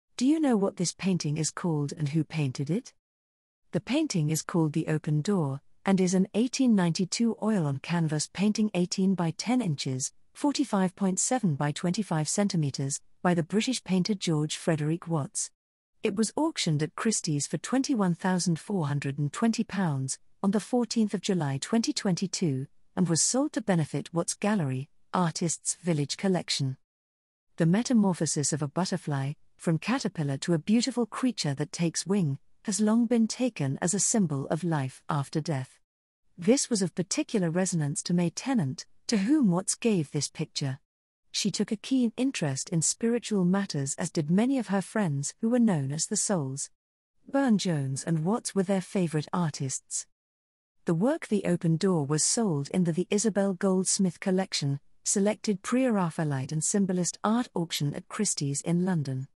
Do you know what this painting is called and who painted it?
The painting is called The Open Door, and is an 1892 oil on canvas painting 18 x 10 in. (45.7 x 25 cm.) by the British painter George Frederic Watts. It was auctioned at Christie's for GBP 21,420, on 14 Jul 2022, and was sold TO BENEFIT WATTS GALLERY - ARTIST’S VILLAGE collection.

The metamorphosis of a butterfly, from caterpillar to a beautiful creature that takes wing, has long been taken as a symbol of life after death. This was of particular resonance to May Tennant, to whom Watts gave this picture. She took a keen interest in spiritual matters as did many of her friends who were known as 'The Souls'. Burne-Jones and Watts were their favourite artists.

The work The Open Door by George Frederick Watts was sold in the The Isabel Goldsmith Collection: Selected Pre-Raphaelite and Symbolist Art auction at Christies in London in July this year. The »bidding war« ended at GBP 21,420 (€ 25,579), 43% above the upper estimate. Of course, this price has nothing to do with the top prices that other works by George Frederick Watts achieve. The highest price we have observed so far was reached by the work Hope in June 2001 with an auction result of GBP 773,750 (€ 1,285,353).